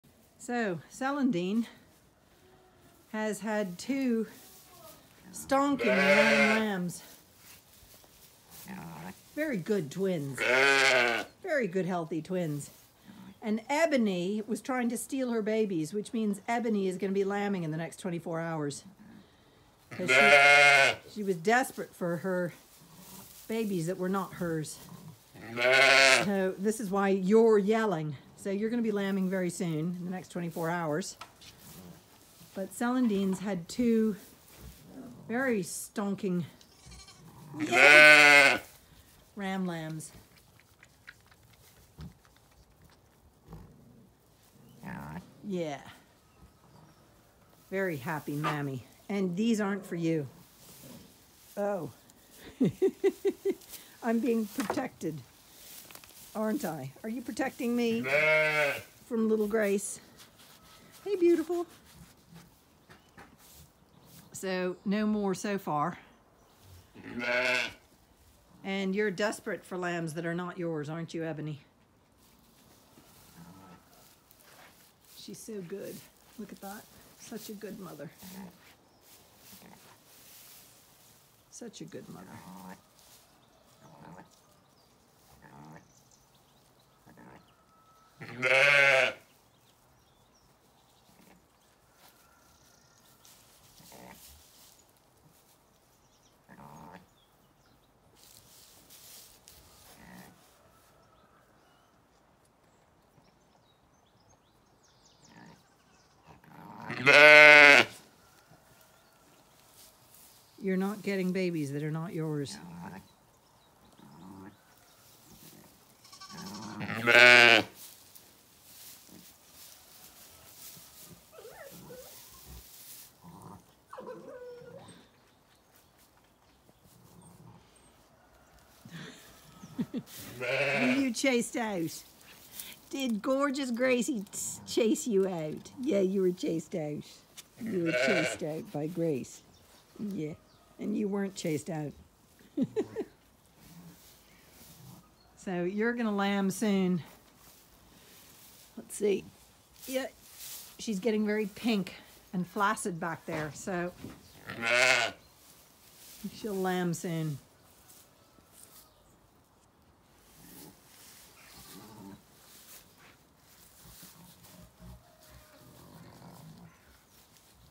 Celandine has twin ram lambs
Ebony thinks I have taken her lamb. This broodiness means she will have lambs sometime soon.